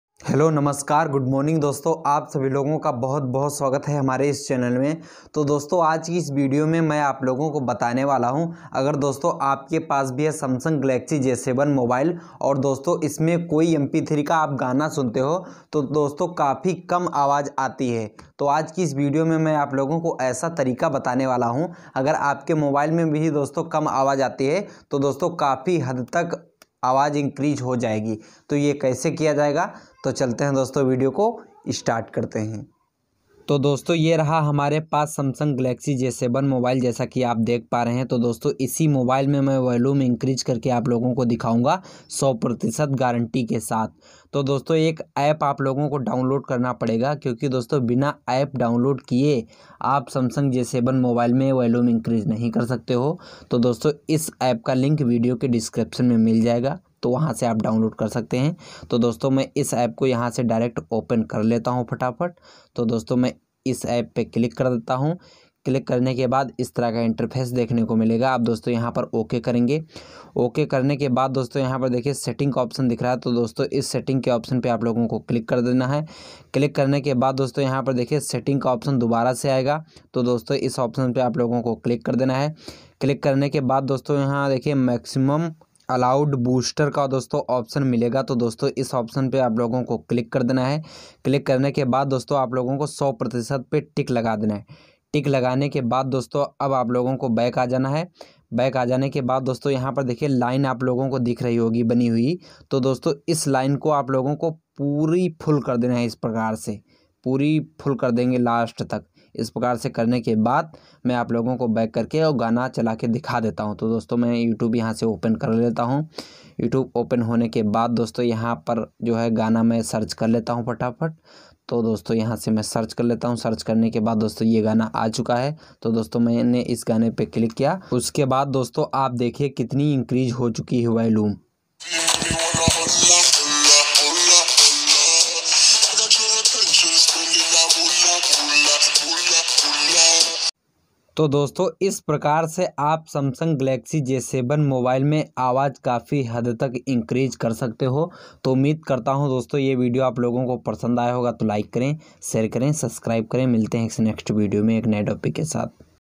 Samsung j7 me volume kaise badhaye | how to increase volume Samsung j7
hello dosto is video me Maine aap logo ko bataya hai hai Samsung Galaxy j7 mobile me volume kaise increase Kare